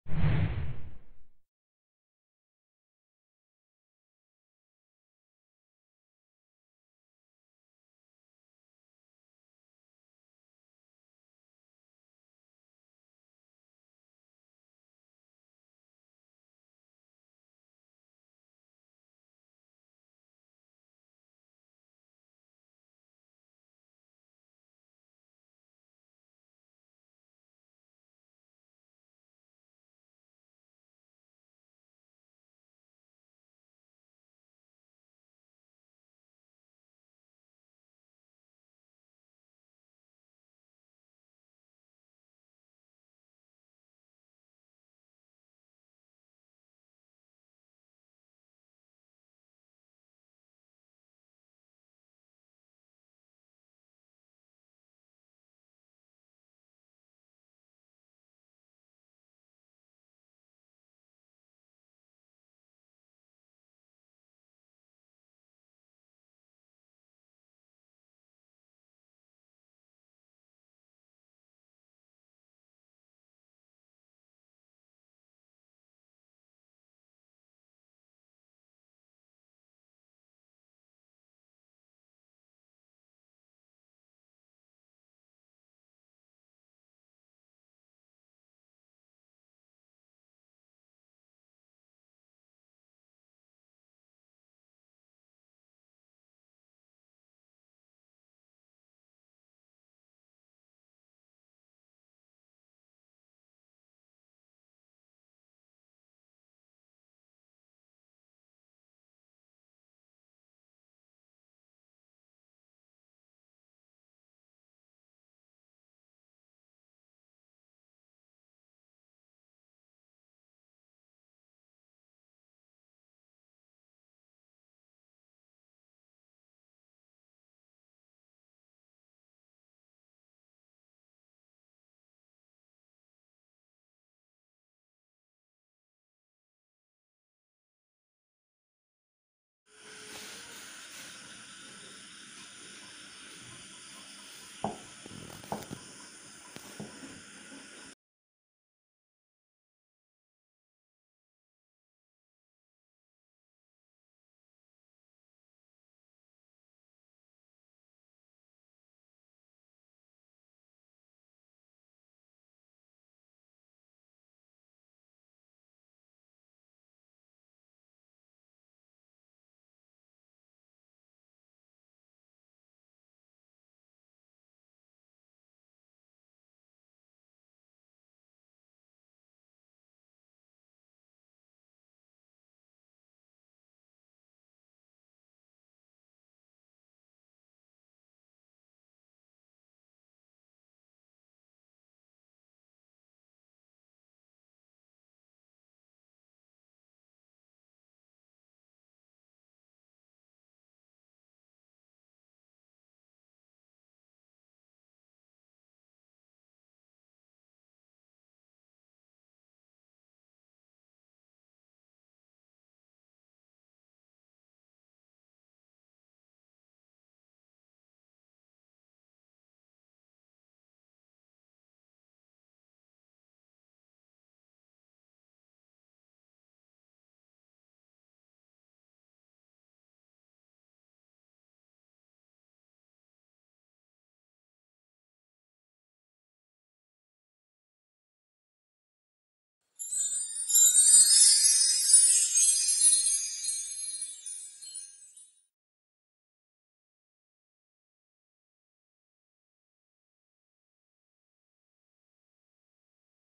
How to make puchka/panipuri at home || without imli || secret ingredient ||Easiest recipe| Sagarika
In this video we showed you how to make Puchkas at home easily.. And how to prepare the pani without imli..